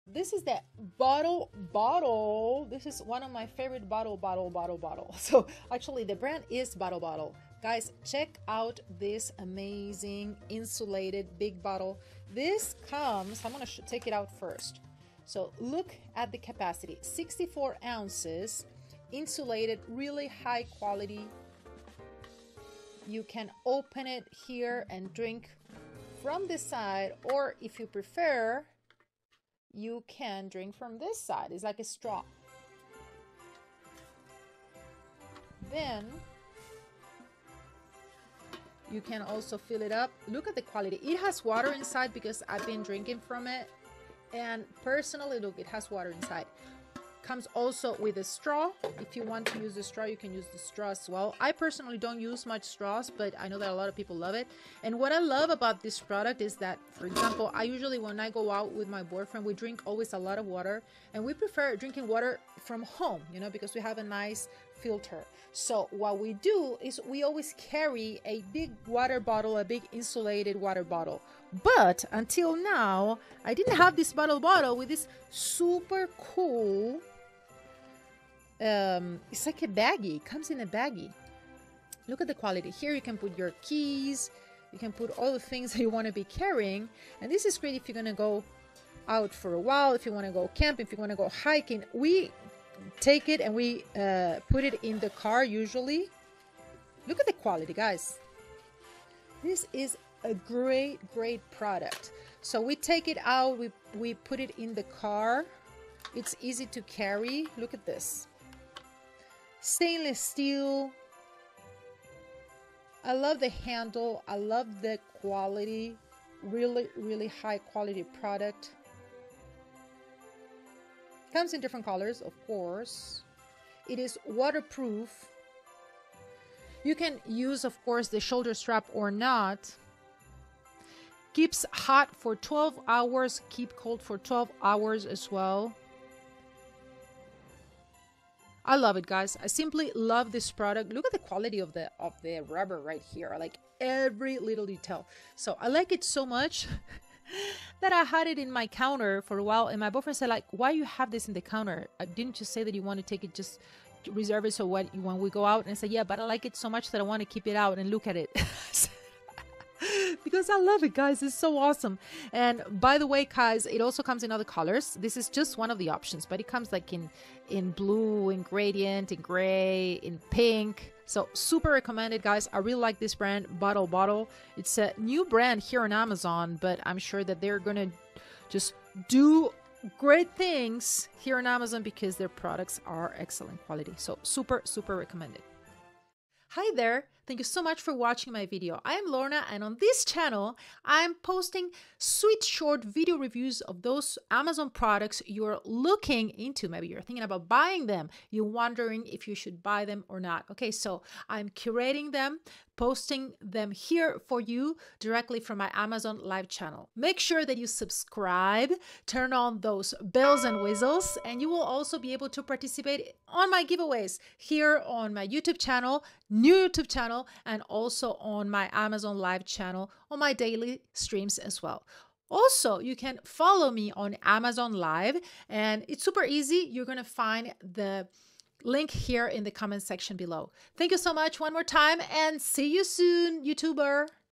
BOTTLE BOTTLE Insulated Water Bottle 64 oz with Straw and Dual-use Lid Half Gallon Water Jug REVIEW
BOTTLE BOTTLE Insulated Water Bottle 64 oz live video review by A-List Amazon Influencer and Singer-songwriter Lorna.

BOTTLE BOTTLE Insulated Water Bottle 64 oz with Straw and Dual-use Lid Half Gallon Water Jug Vacuum Stainless Steel for Workout and Sports Insulated Beer Growler with Handle（Black）

Find this BOTTLE BOTTLE Insulated Water Bottle 64 oz on Amazon